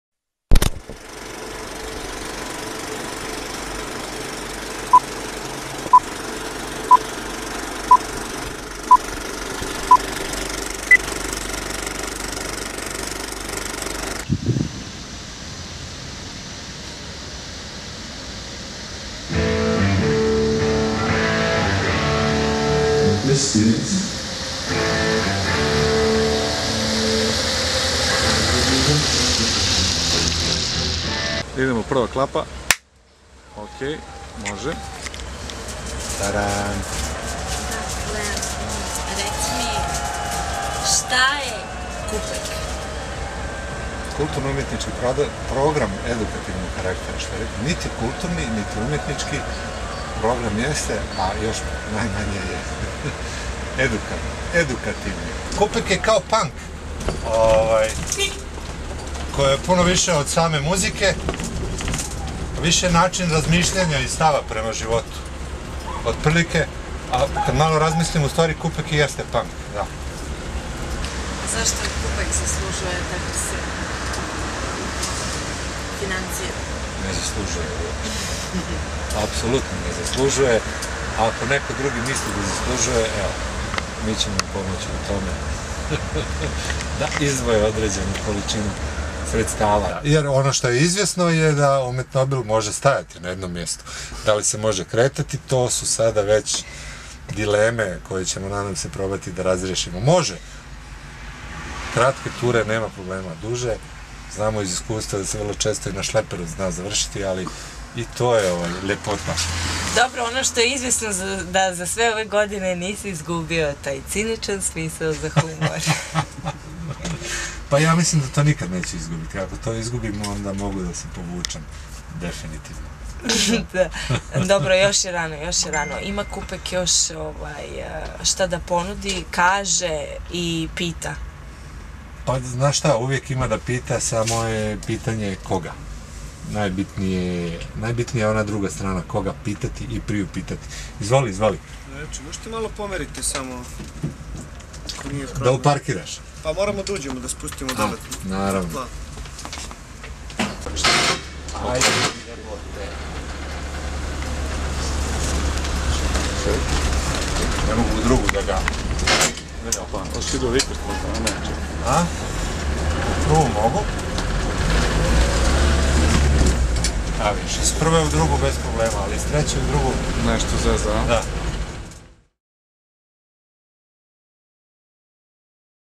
Zašto (ne) podržati KUPEK II dio
Jedna od specifičnosti KUPEK-a je Umetnobil, "vozilo za višenamensko delovanje". Topla letnja beogradska noć činila se idealnom scenografijom za novi video za KUPEK crowdfunding kampanju. Zahvaljujući Zuletu i Luciji.